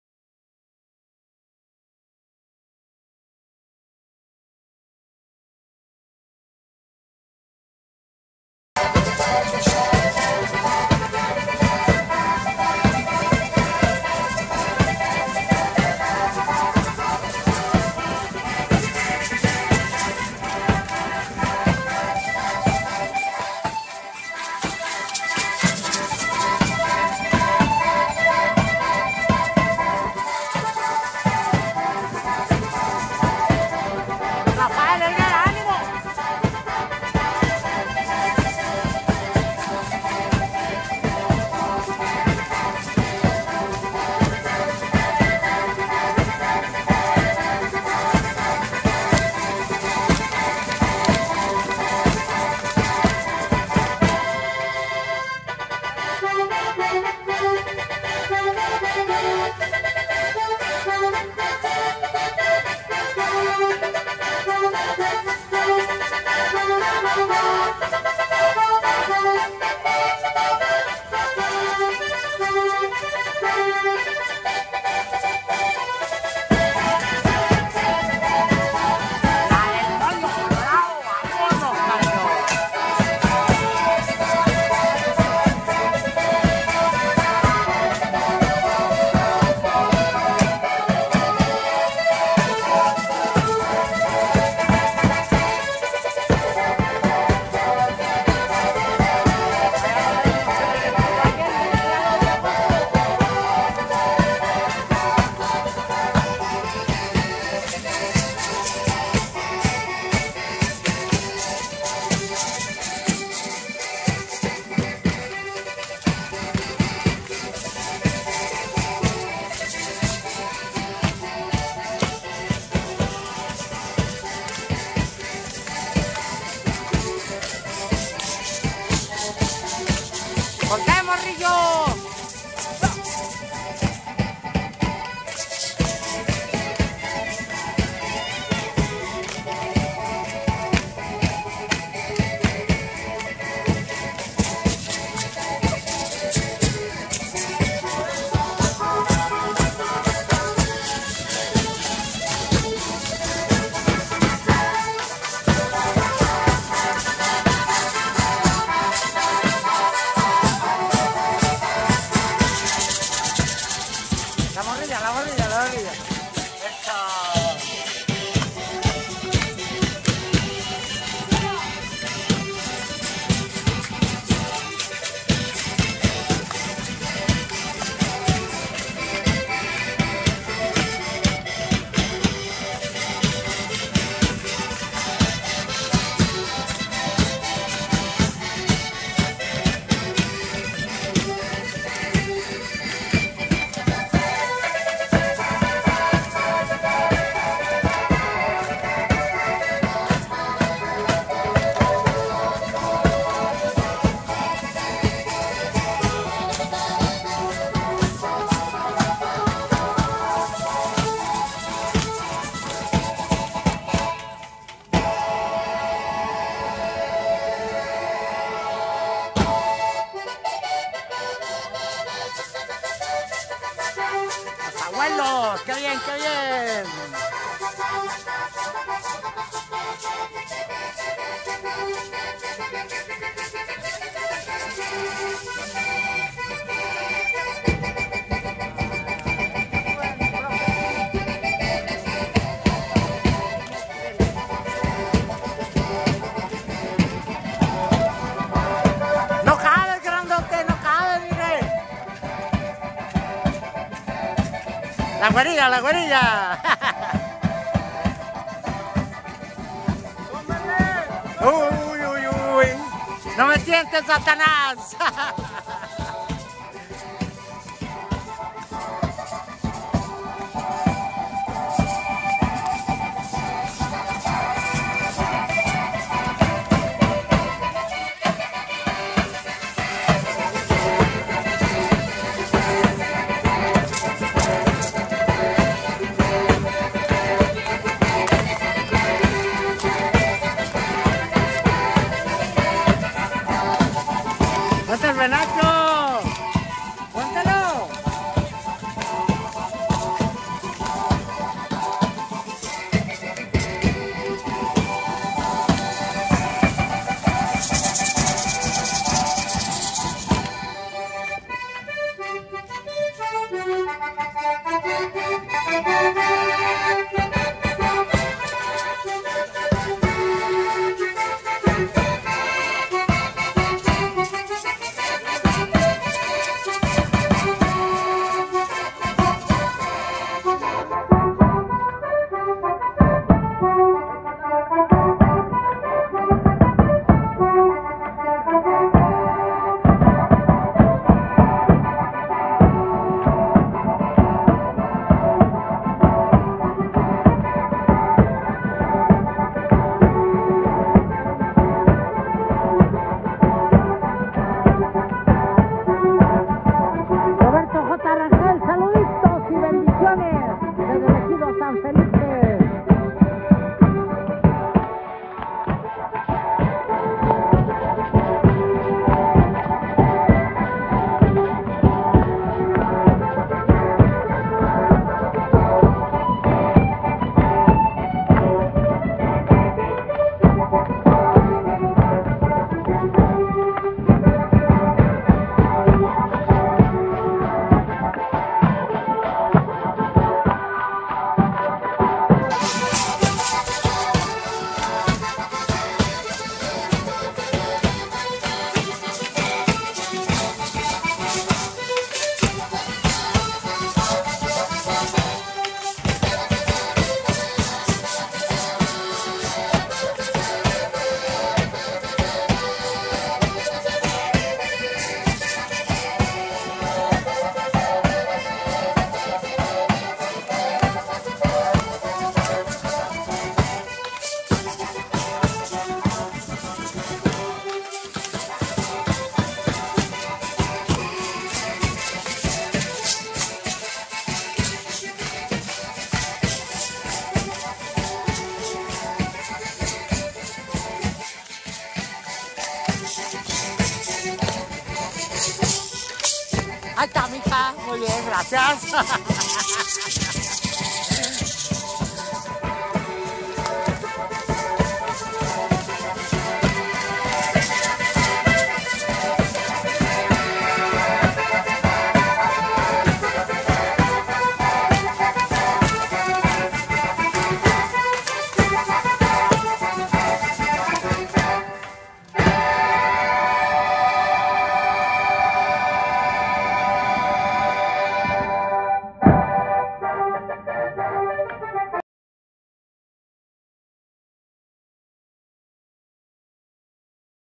"Quetzales del Divino Niño" de San Felipe/El Compás, Dgo.
Fiesta del Divino Niño en San Felipe, Gómez Palacio, Dgo. 24 de julio 2022.
+Danza “Los Quetzales del Divino Niño” de San Felipe/El Compás, Dgo.